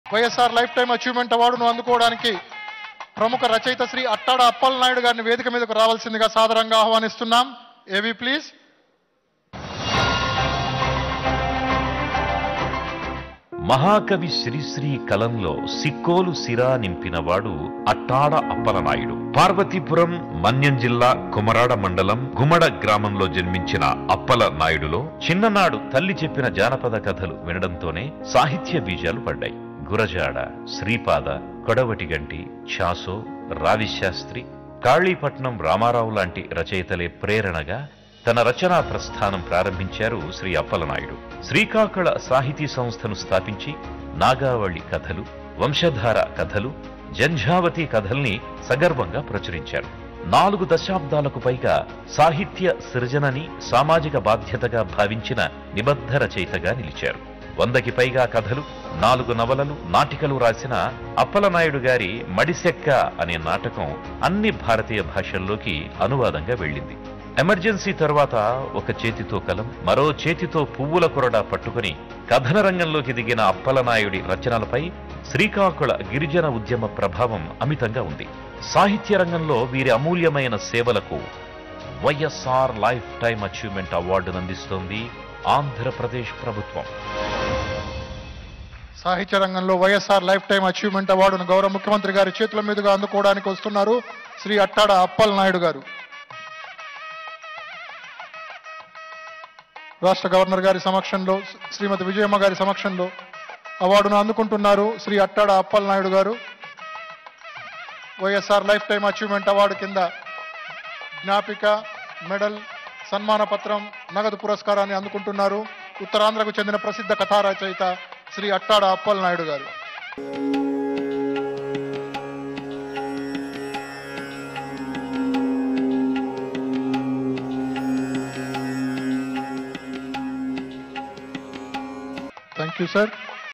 Writer Attada Appalanaidu Receives YSR Lifetime Achievement Award 2023 | CM Jagan
Writer Attada Appalanaidu Receives YSR Lifetime Achievement Award 2023 | CM Jagan | YS Vijayamma | @SakshiTV